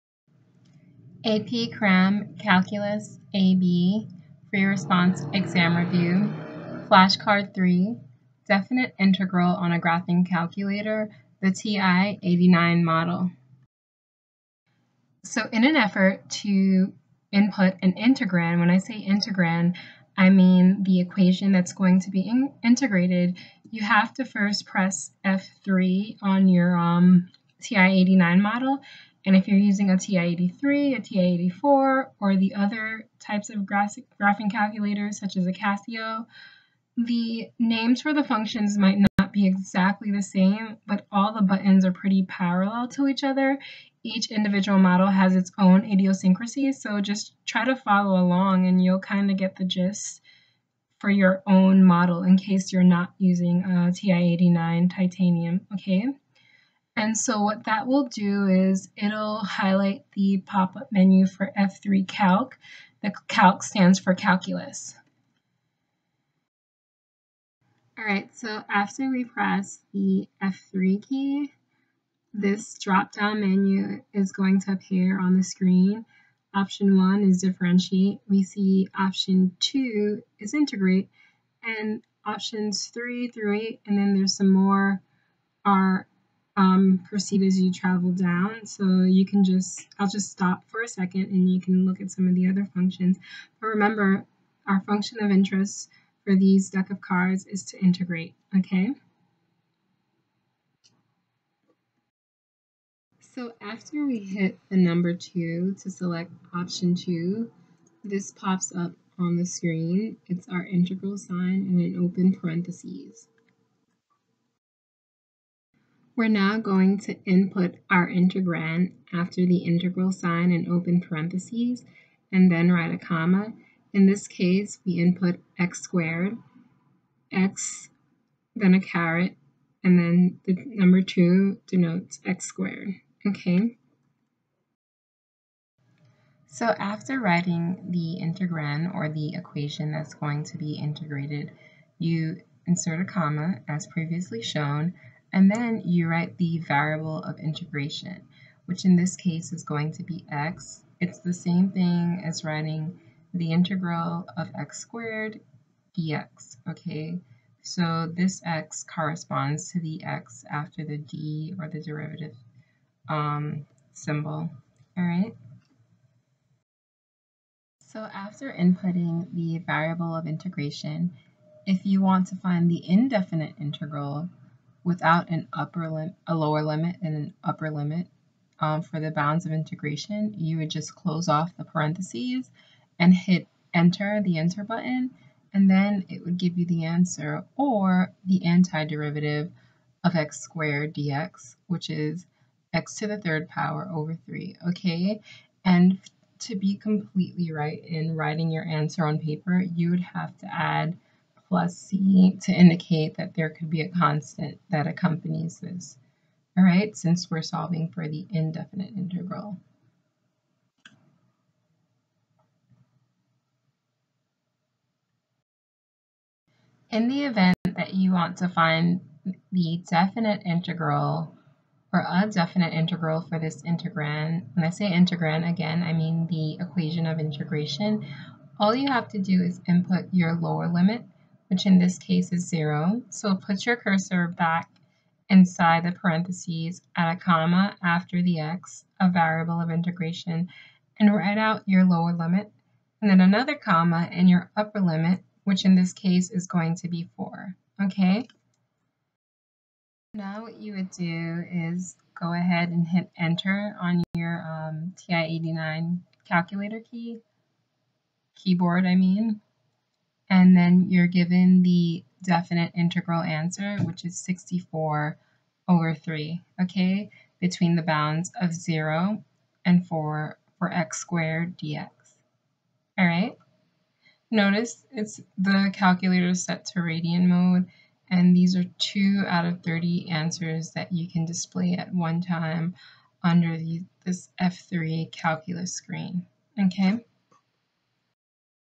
AP® Cram│ AP Calculus Exam │Free Response Flashcard 3│ Evaluate a Definite Integral on a TI 89
AP® Cram│ AP Calculus Exam │Free Response Flashcard 3│ Evaluate a Definite Integral  on a graphing calculator (TI 89)

Flashcard 3 -The AP Calculus Exam Review│ Evaluate a Definite Integral │ On a graphing calculator (T1-89)

These flashcards:
Details how to solve a definite integral on a graphing calculator